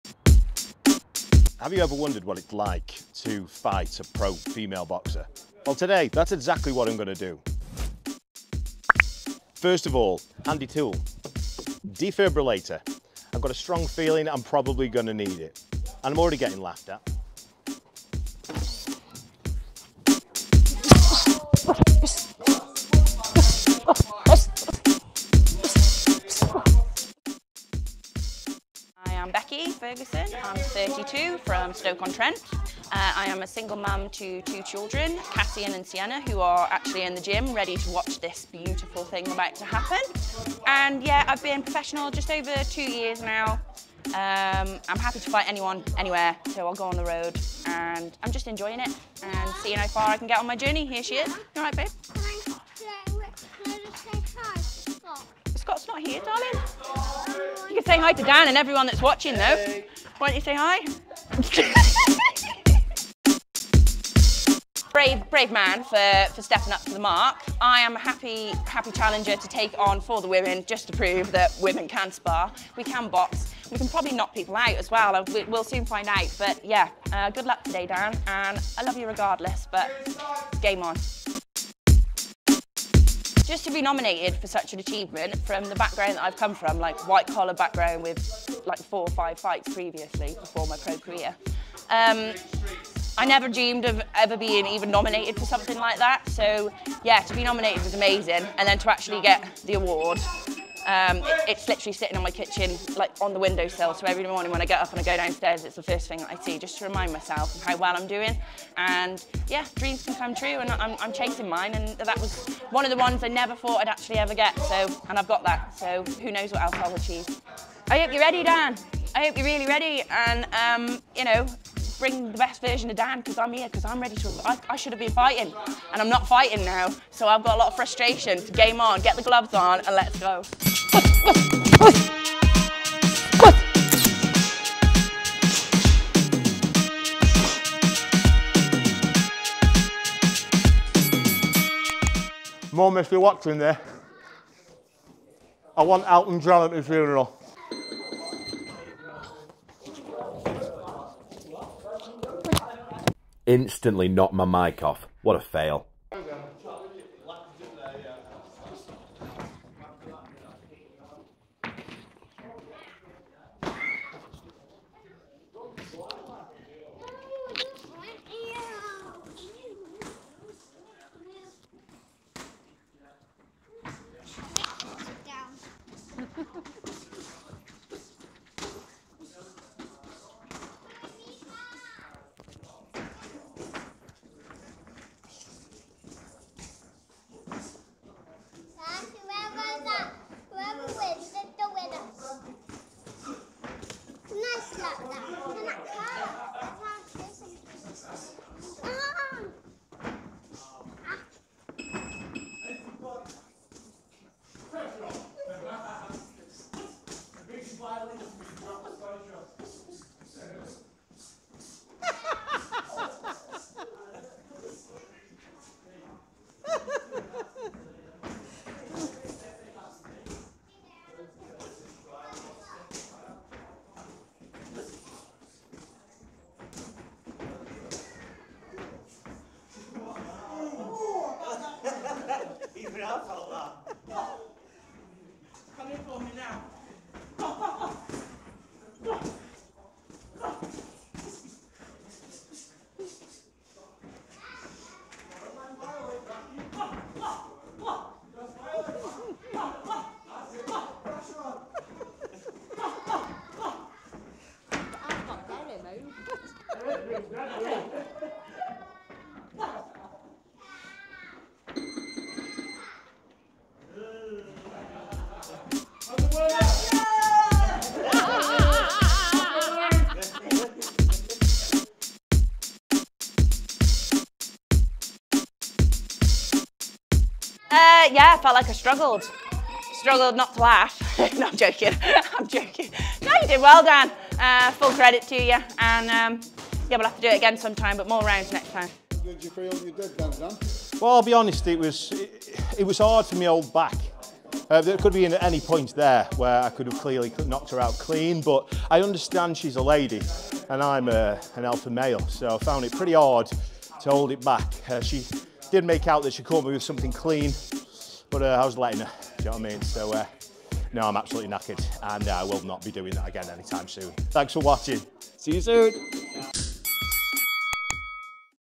I FOUGHT A PROFESSIONAL FEMALE BOXER
Dan Hewitt at Boxing TV jumps in to the ring with female professional boxer Beccy Ferguson. How did the big man do against the Midlands area title challenger?